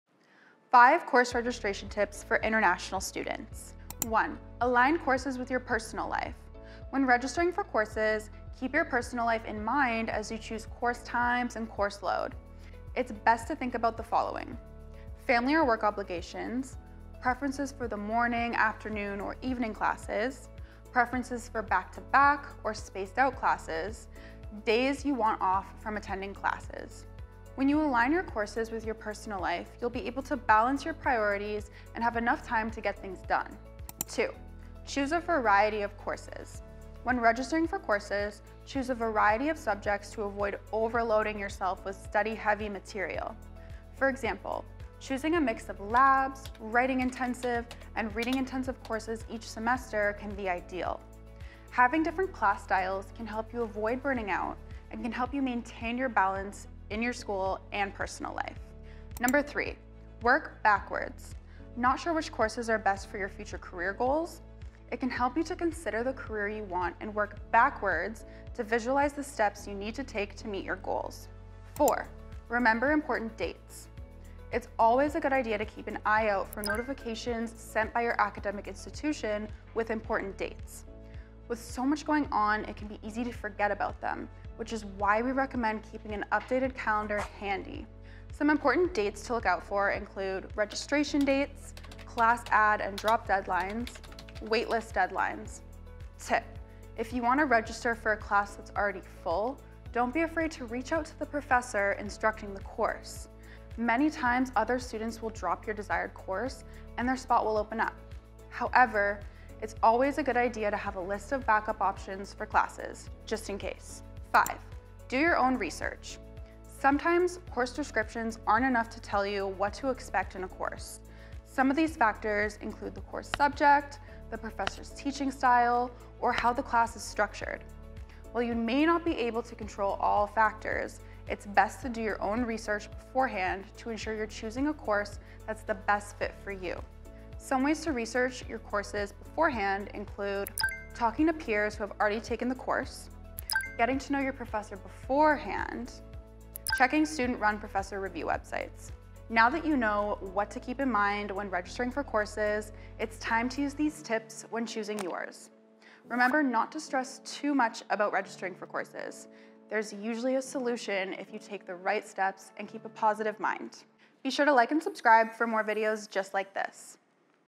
How To Choose College Classes + Course Registration Tips!
Picking your classes doesn't need to be scary! We've got 5 course registration tips to prepare you for success in your upcoming school year.

Remember: Every school is different! So be sure to thoroughly read any communications you receive from your school—they'll be sending you all the info you need about registration dates and deadlines.

👇 JUMP TO SECTIONS:
0:00 Intro
0:04 Aligning courses with your personal life
0:34 Choosing a variety of courses
0:59 Working backwards
1:12 Important dates
1:54 Doing research
2:29 Outro